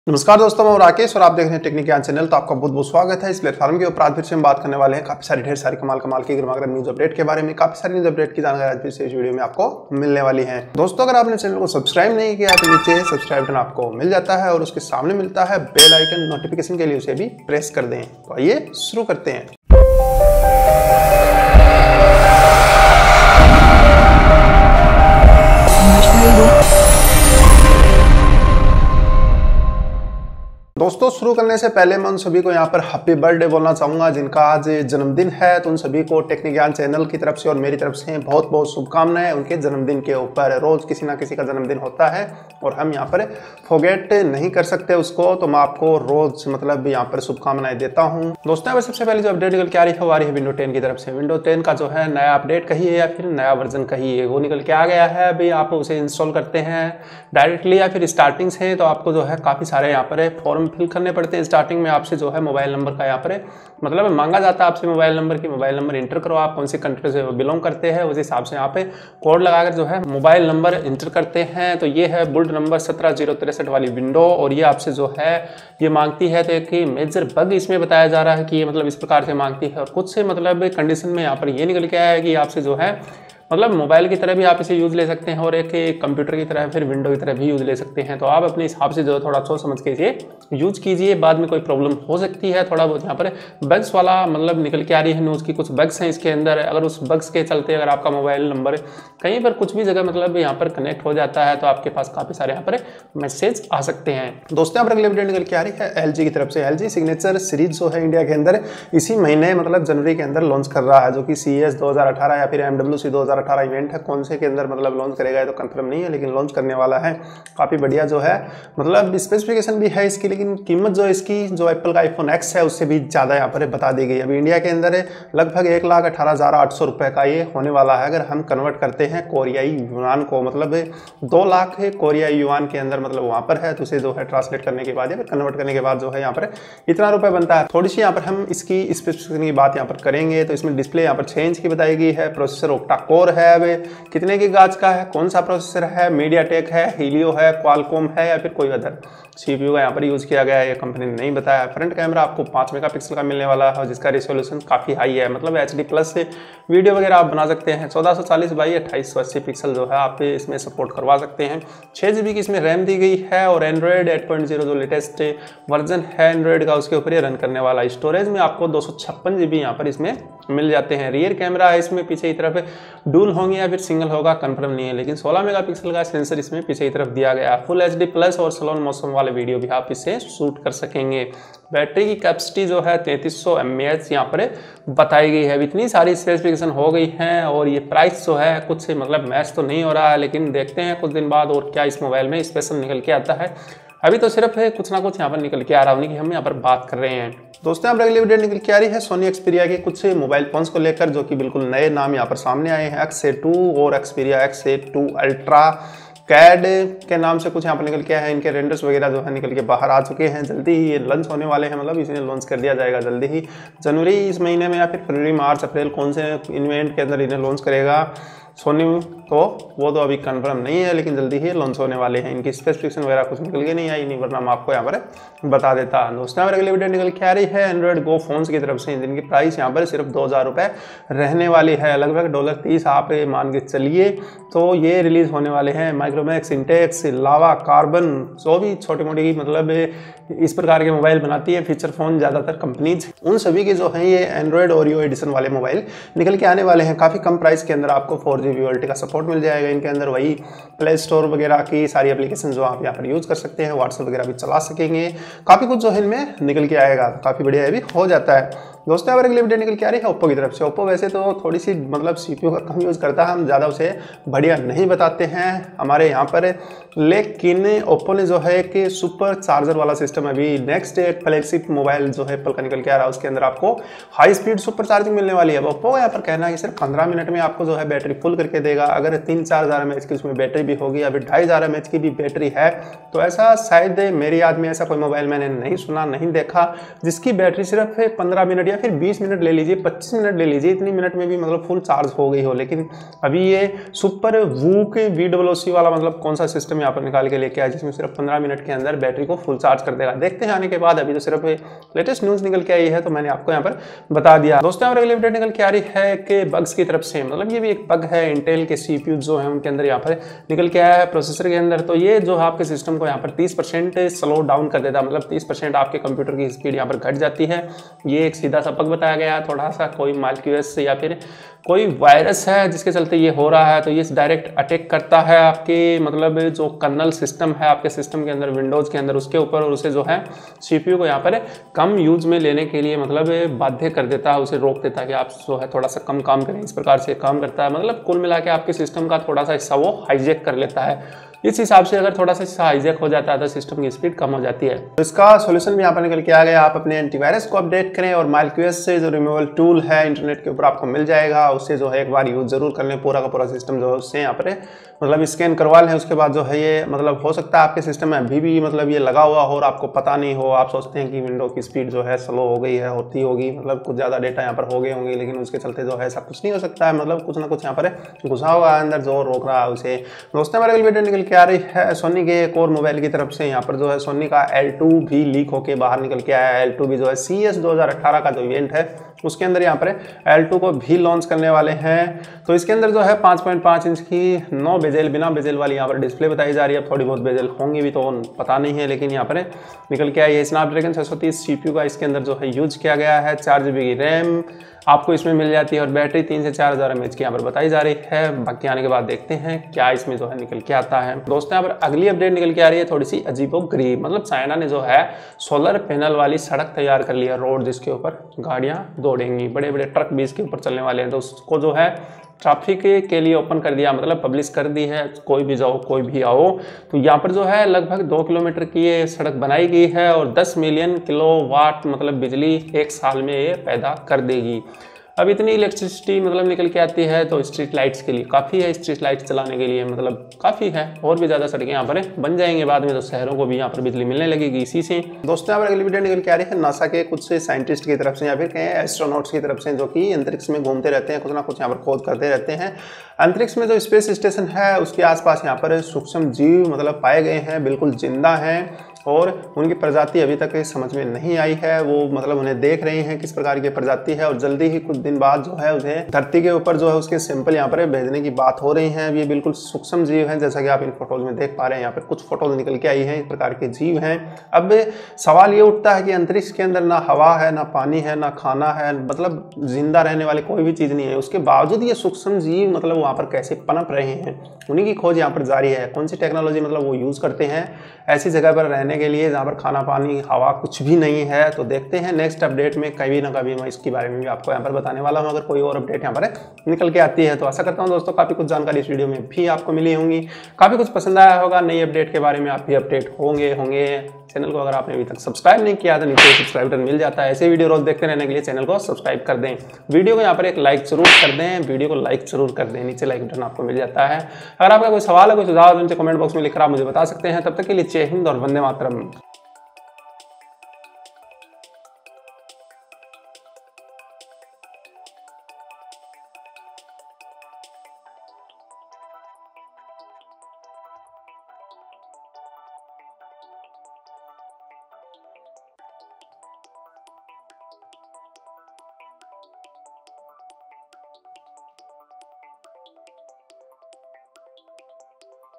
Tech Up's #111- Solar Road, Space Microbes, Xperia L2, XA2, Fast Oppo, CPU Hijeck, LG Signature
HINDI - Tech Up's #111- Solar Road, Space Microbes, Xperia L2, XA2, Fast Oppo, CPU Hijeck, LG Signature

1. Latest Windows 10 Build Won't Install Without Your Phone Number
  Build No. 17063 It Have a mazer Bug
2. LG Signature Series to Launch in India on Friday
     कीमत 2,000,000 कोरियाई वॉन (करीब 1,18,800 रुपये) है
डिस्प्ले 6.00 इंच -प्रोसेसर ऑक्टा-कोर - फ्रंट कैमरा 5-मेगापिक्सल -  रिज़ॉल्यूशन 1440x2880 पिक्सल - रैम6 जीबी
ओएस एंड्रॉ़यड 8.0    स्टोरेज 256 जीबी     रियर कैमरा 16-मेगापिक्सल    बैटरी क्षमता 3300 एमएएच
3. Sony Xperia XA2, Xperia XA2 Ultra CAD Renders Appear as Launch Nears
4. Android Go Phones Priced Around Rs. 2,000 (~$30) to Release in India This Month
Micromax, Intex, Lava, Karbonn and others to release Android Go (Android Oreo edition)
5. OPPO Next Flagship Phone May Feature Super VOOC, Charger Full in Just 15 Minutes
6. Fixing this Serious Intel Processor Bug will Slow Down your Copmuter
Speed Down About 30% -- Bug is in Kernel System – Hijeck Your System
7. Upcoming Sony Xperia L2 Leaked in Exclusive 3D Renders
5.5” No Bezel, SD 630 CPU, 4GB RAM, Launch At CES 2018
8. China's First Solar Highway is Now Open to Traffic
 2KM – 10Million KWH Electricity Per Year
9. ISS Astronauts Have Identify Unknown Microbes in Space For the First Time
आईएसएस अंतरिक्ष यात्री पहले समय के लिए अंतरिक्ष में अज्ञात सूक्ष्मजीवों की पहचान करते हैं